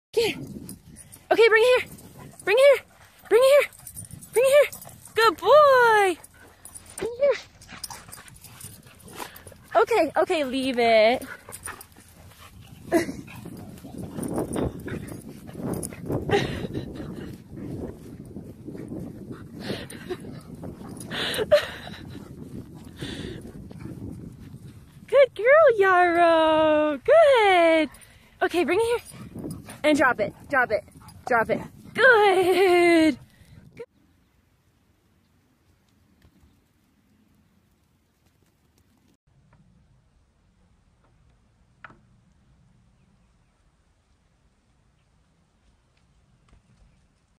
Shepherd Dog Walks Wild Mustang By Leash (Storyful, Dogs)
Yarrow the mustang had a colorful past before coming into Sierra’s care. The two were united when Sierra chose Yarrow back in February of this year in order to compete in the Colorado Extreme Mustang Makeover. The competition involved training a wild mustang to be tame within 100 days. Yarrow certainly demonstrates how tame she is by letting Elyot, Sierra’s Australian shepherd, lead her around. Apparently she also lets Elyot climb onto her back!